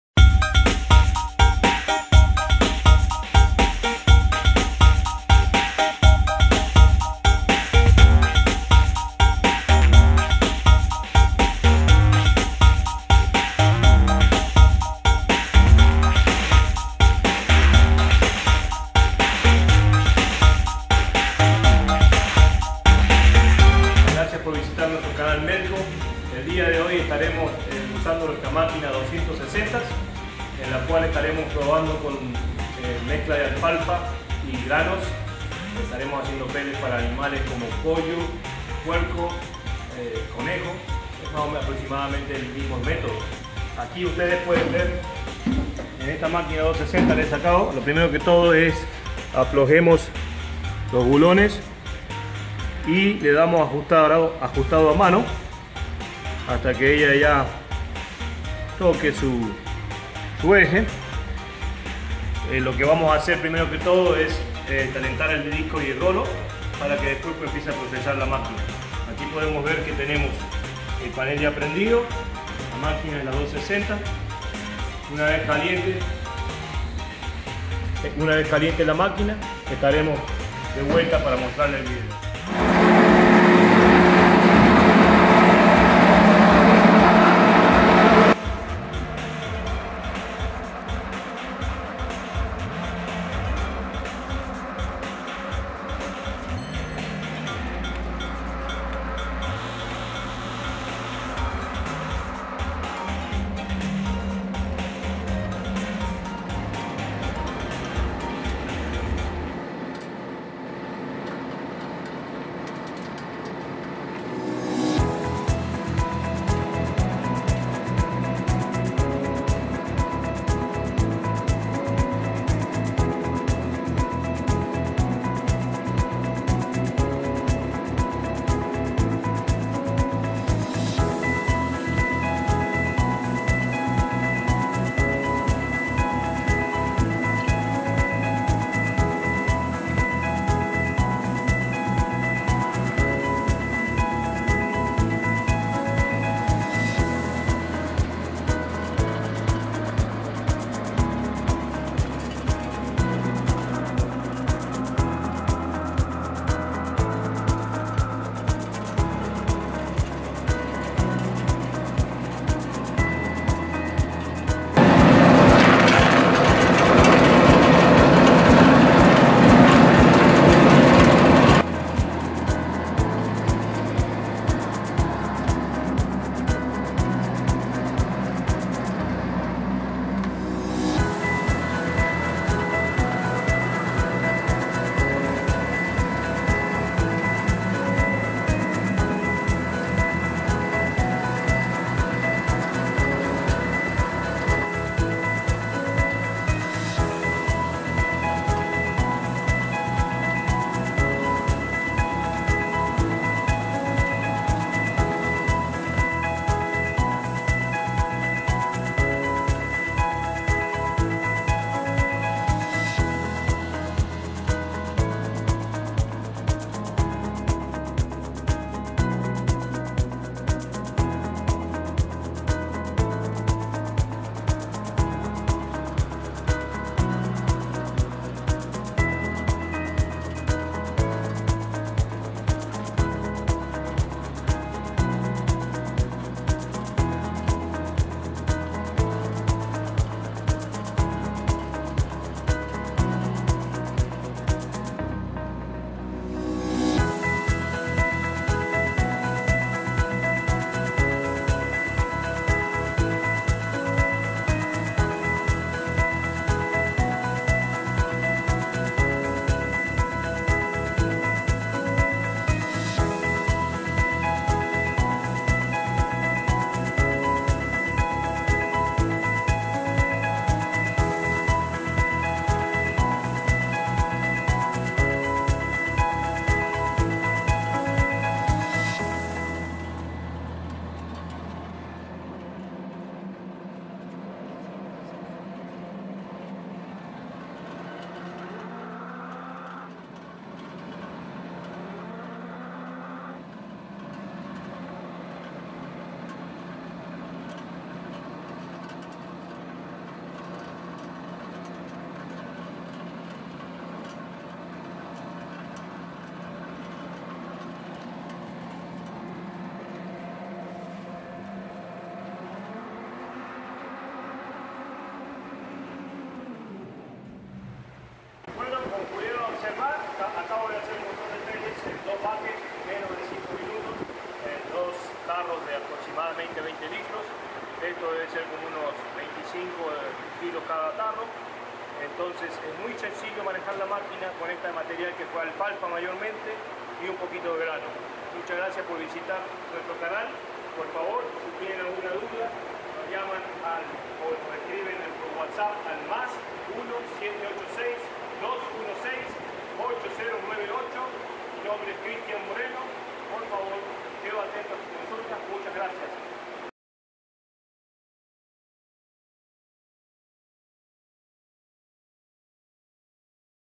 MEELKO - Prensado de alfalfa en pellet alimento de ganado.
Utilización de peletizadora de alfalfa y cereales para alimento balanceado de ganado mayor y menor.